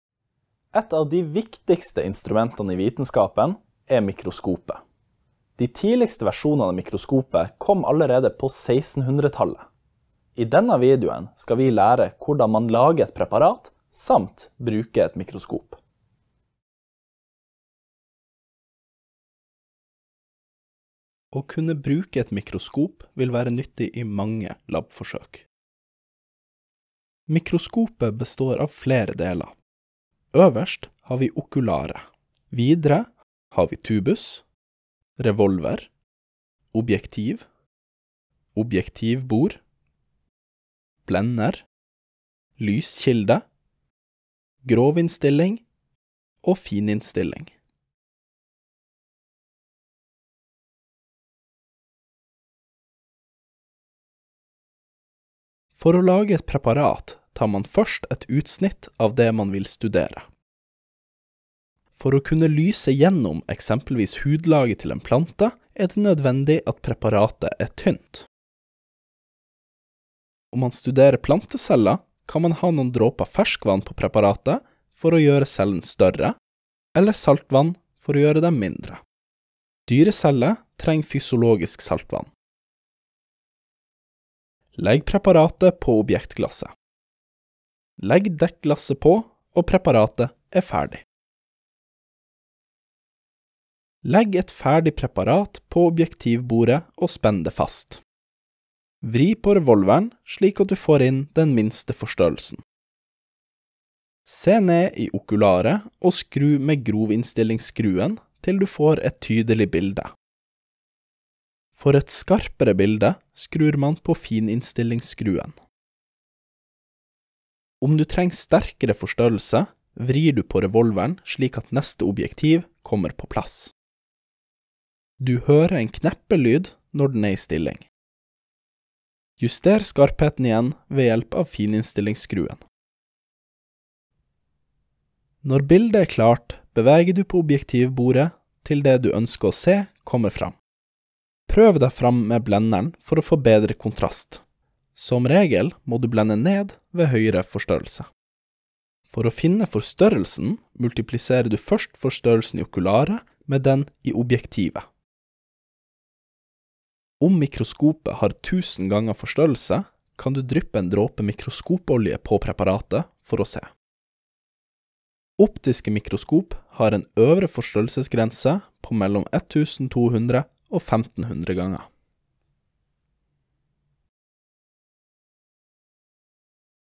Labforsøk Biologi 1: Mikroskop
Demonstrasjon av hvordan man bruker et mikroskop, samt hvordan lage et preparat. Biologi 1 gir deg 0,5 realfagspoeng og skal du studere medisin bør du ha biologi